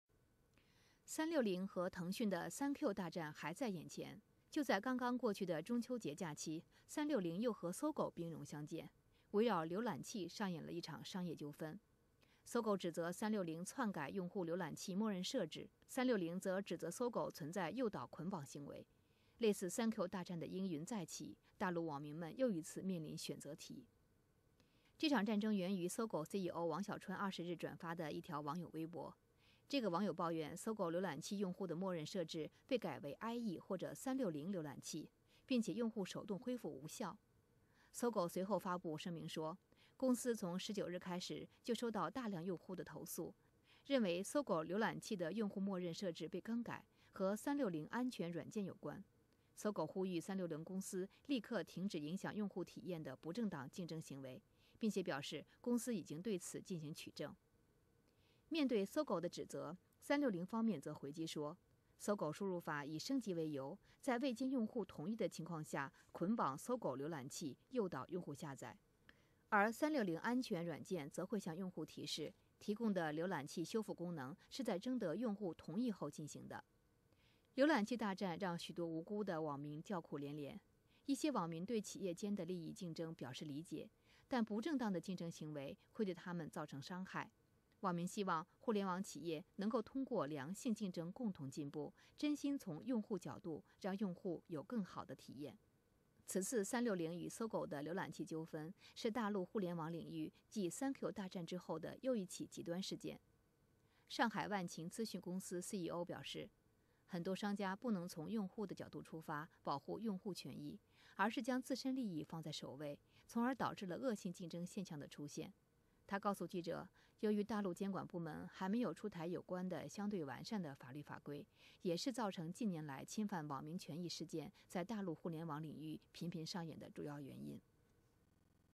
新華社》搜狗爆發瀏覽器大戰 線民無辜被綁架
這場戰爭緣起於搜狗CEO王小川20日轉發的一條網友微博。這個網友抱怨搜狗流覽器用戶的默認設置被改為ＩＥ或360瀏覽器，並且用戶手動恢復無效。